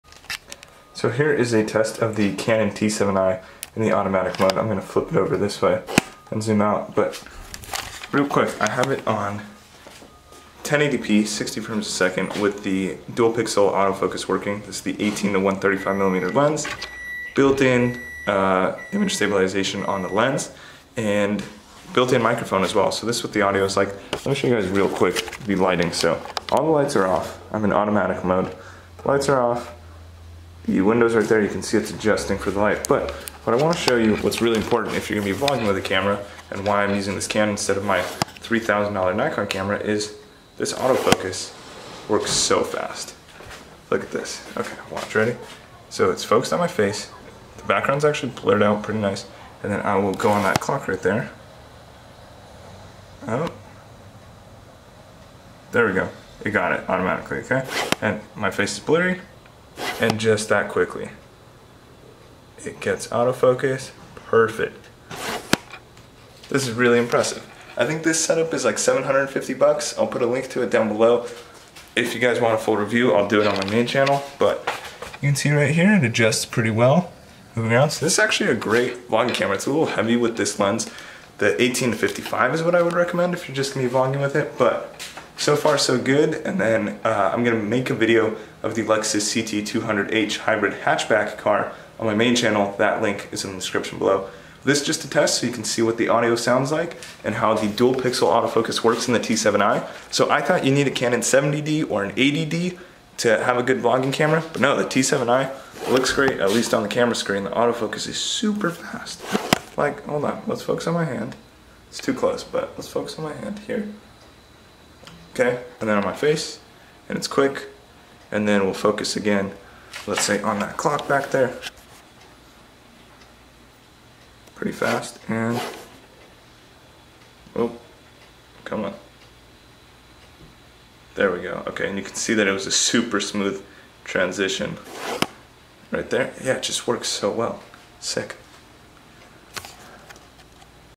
Canon T7i - Vlogging Autofocus Test (Dual Pixel AF)
This T7i

I'm genuinely impressed with both the audio quality of the built in microphone and the autofocus on the "budget" camera that is the Canon T7i. As you can see in this video it does a great job of autofocusing on my face for vlog style shooting with the 18-135mm IS kit lens.